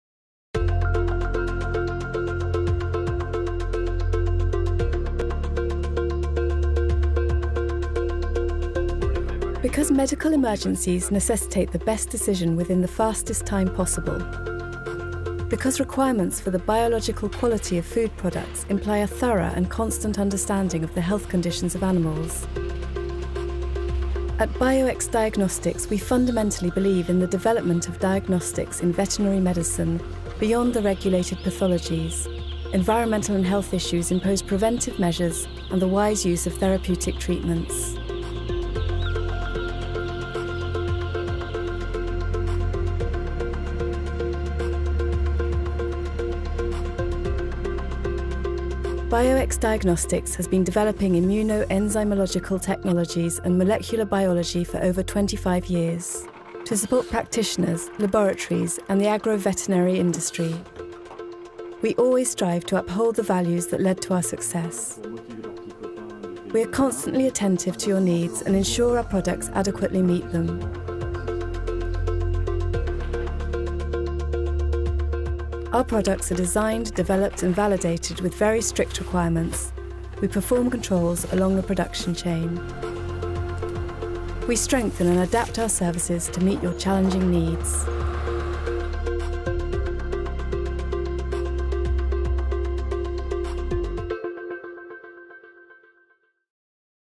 Presentation of our values
Bio-X Diagnostics has  been developing immuno-enzymological technologies and molecular biology for over 25 years,
To support practitioners, laboratories and the agro-veterinary industry.

We always strive to uphold the values that led to our success.

We are constantly attentive to your needs and ensure our products adequately meet them.